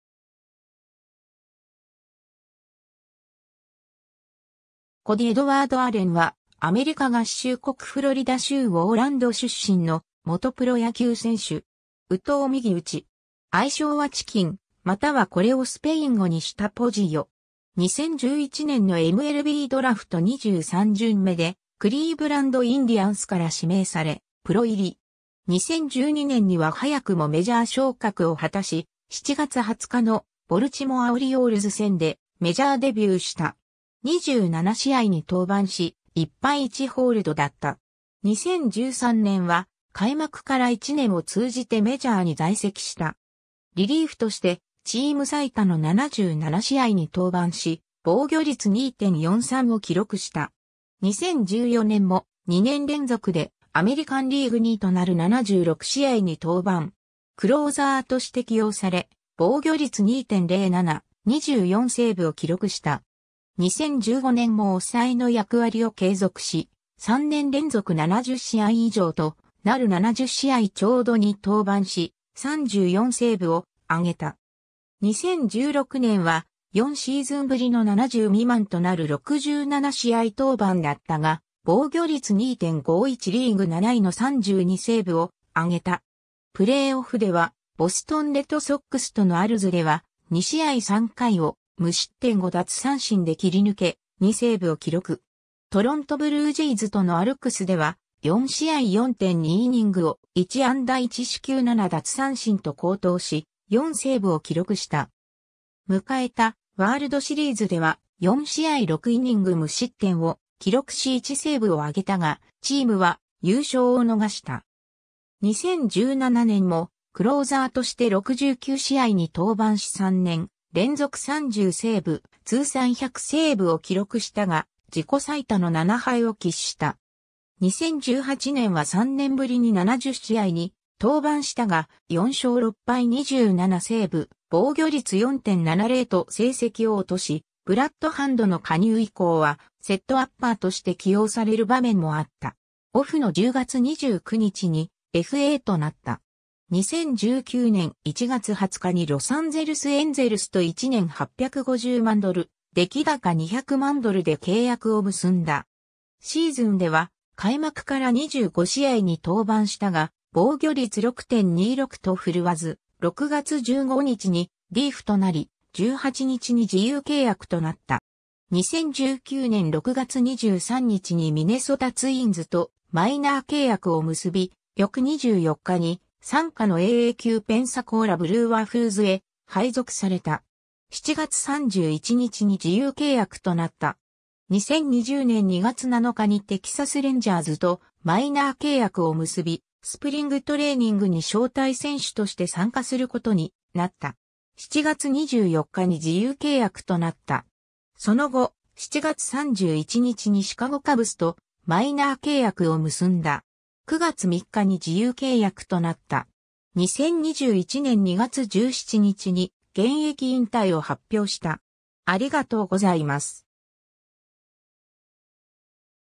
コディ・アレン
コディ・エドワード・アレン（Cody Edward Allen, 1988年11月20日 - ）は、アメリカ合衆国フロリダ州オーランド出身の元プロ野球選手（投手）。
右投右打。
 愛称はチキン、またはこれをスペイン語にしたポジョ（Pollo）。
 2011年のMLBドラフト23巡目（全体698位）でクリーブランド・インディアンスから指名され、プロ入り。
 2012年には早くもメジャー昇格を果たし、7月20日のボルチモア・オリオールズ戦でメジャーデビューした。
27試合に登板し、1敗1ホールドだった。
 2013年は開幕から一年を通じてメジャーに在籍した。
リリーフとしてチーム最多の77試合に登板し、防御率2.43を記録した。
 2014年も、2年連続でアメリカンリーグ2位となる76試合に登板。
クローザーとして起用され、防御率2.07、24セーブを記録した。
 2015年も抑えの役割を継続し、3年連続70試合以上となる70試合ちょうど（リーグ9位タイ）に登板し、34セーブ（リーグ6位タイ）を挙げた。
 2016年は、4シーズンぶりの70未満となる67試合登板だったが、防御率2.51・リーグ7位の32セーブを挙げた。
プレーオフではボストン・レッドソックスとのALDSでは、2試合・3回を無失点5奪三振で切り抜け、2セーブを記録。
トロント・ブルージェイズとのALCSでは、4試合・4.2イニングを1安打1四球7奪三振と好投し、4セーブを記録した。
迎えたワールドシリーズでは、4試合・6イニング無失点を記録し1セーブを挙げたが、チームは優勝を逃した。
 2017年もクローザーとして69試合に登板し3年連続30セーブ、通算100セーブを記録したが、自己最多の7敗を喫した。
 2018年は3年ぶりに70試合に登板したが、4勝6敗27セーブ、防御率4.70と成績を落とし、ブラッド・ハンドの加入以降はセットアッパーとして起用される場面もあった。
 2019年1月20日にロサンゼルス・エンゼルスと1年850万ドル+出来高200万ドルで契約を結んだ。
シーズンでは開幕から25試合に登板したが防御率6.26と振るわず、6月15日にDFAとなり、18日に自由契約となった。
 2019年6月23日にミネソタ・ツインズとマイナー契約を結び、翌24日に傘下のAA級ペンサコーラ・ブルーワフーズへ配属された。
7月31日に自由契約となった。
 2020年2月7日にテキサス・レンジャーズとマイナー契約を結び、スプリングトレーニングに招待選手として参加することになった。
7月24日に自由契約となった。
 その後、7月31日にシカゴ・カブスとマイナー契約を結んだ。
9月3日に自由契約となった。
 2021年2月17日に現役引退を発表した。